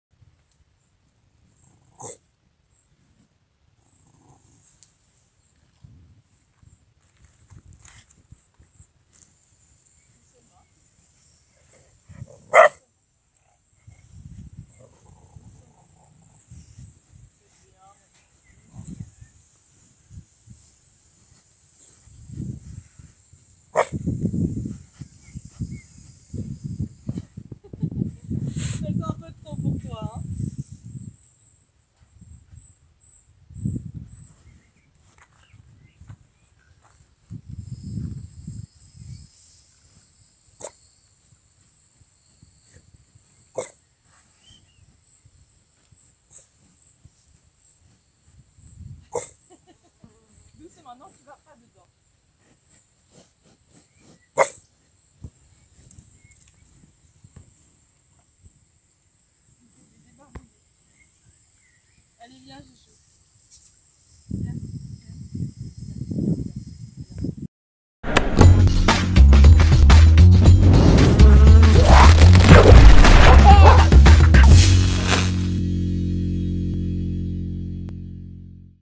Cows absolutely fascinated by bulldog's presence
James the very sweet bulldog has made some new friends in this adorable clip. The cows just can't get enough of him!